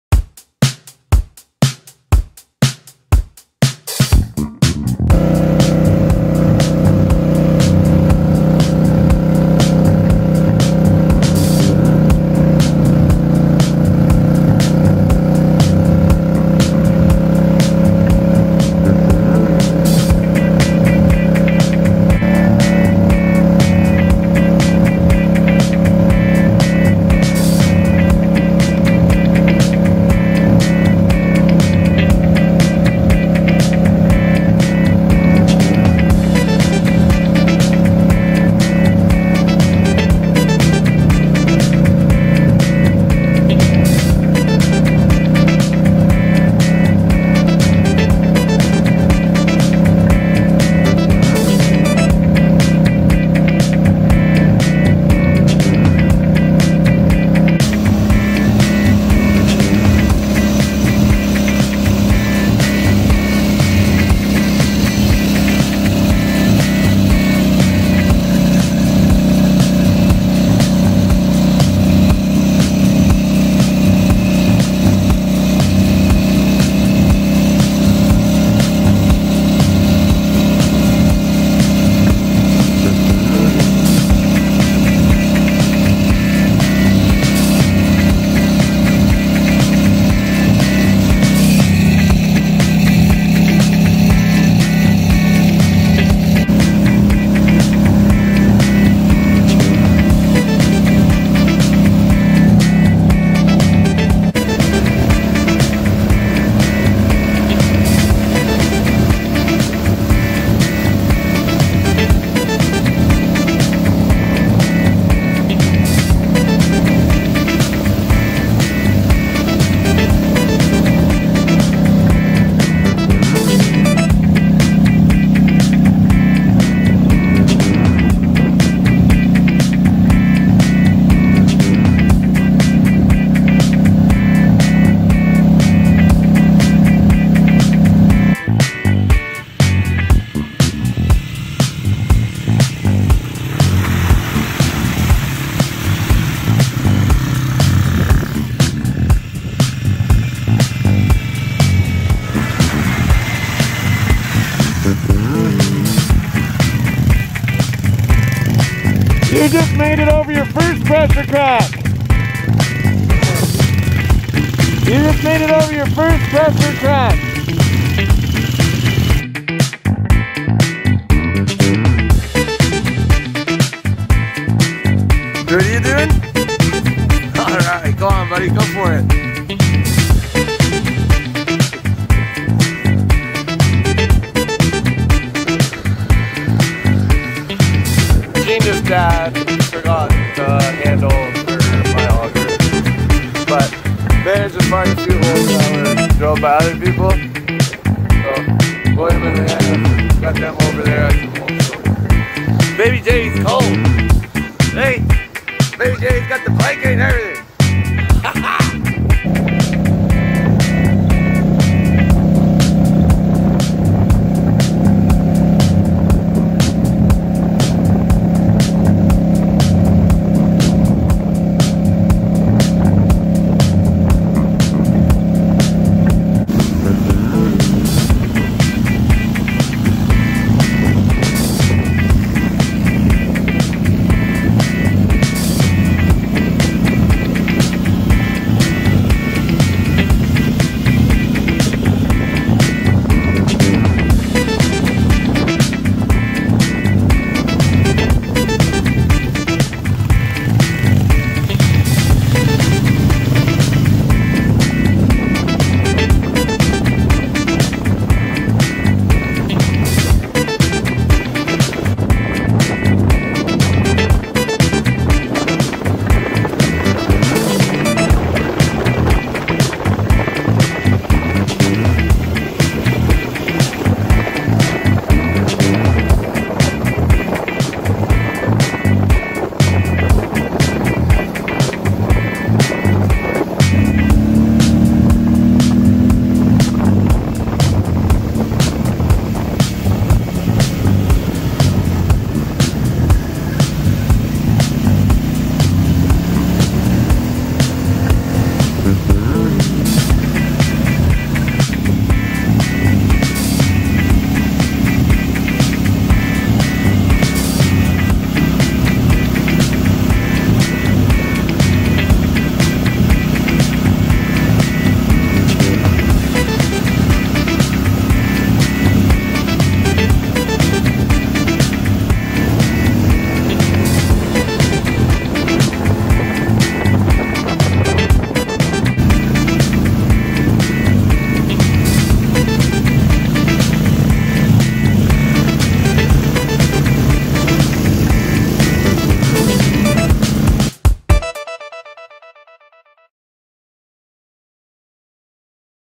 River run
Team Cotter Kids made the journey from RDP all the way out to lake simcoe for an awesome day of riding and a lil ice fishing!